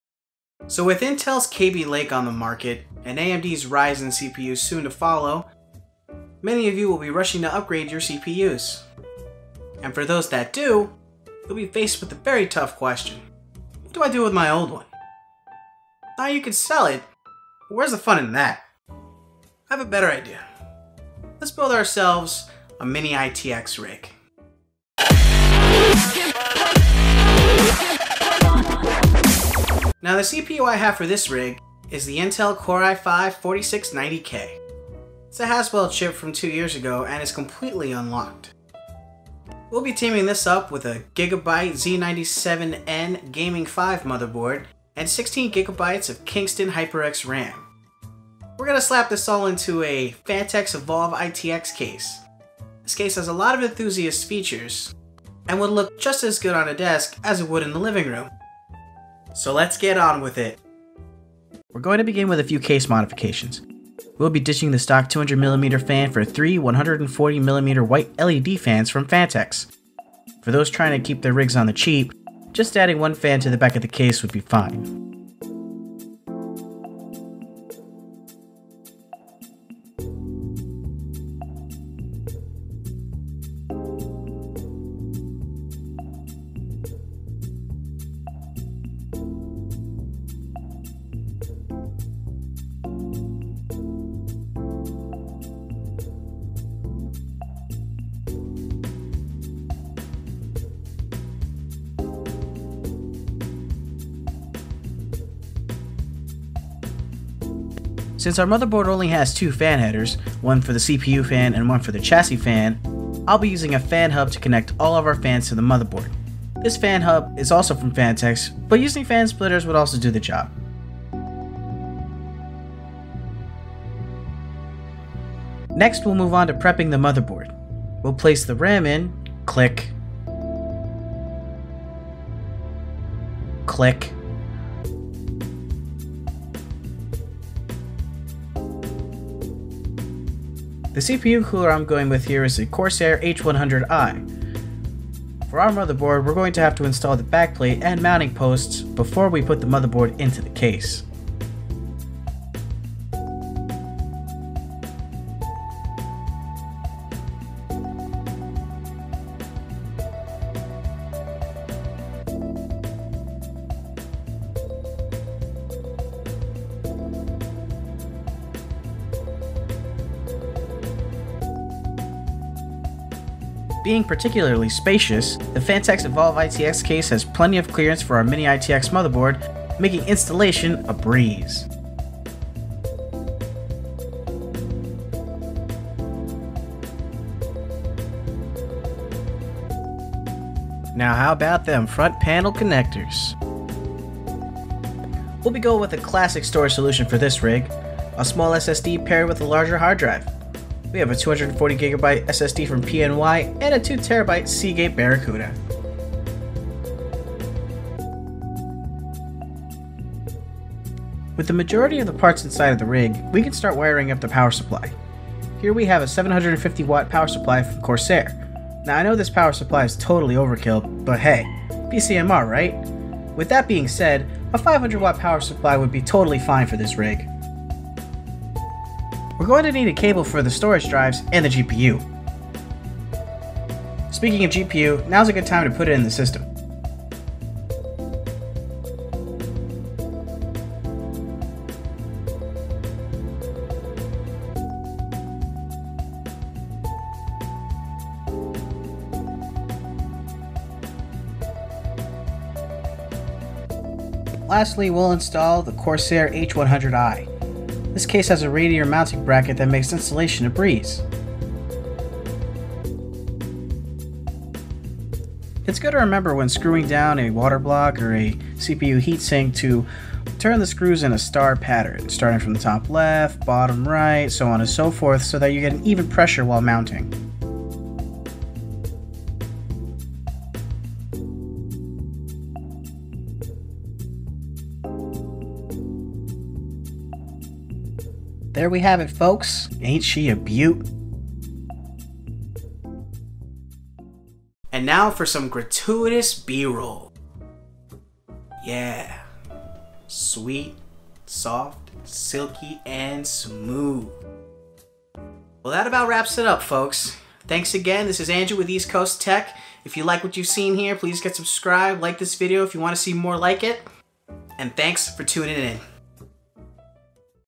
What To do With An Old CPU?
To sell or not to sell, that is the question. With an influx of new CPU chips on the market, can that old chip you have lying around get a second wind?

Intel Core i5 4690k
Gigabyte z97 Replacement
Kingston HyperX Fury Red
EVGA GTX 980
Corsair H100i V2
PNY CS2211 240GB SSD
Seagate Desktop HDD 2TB
Phanteks Evolv ITX Red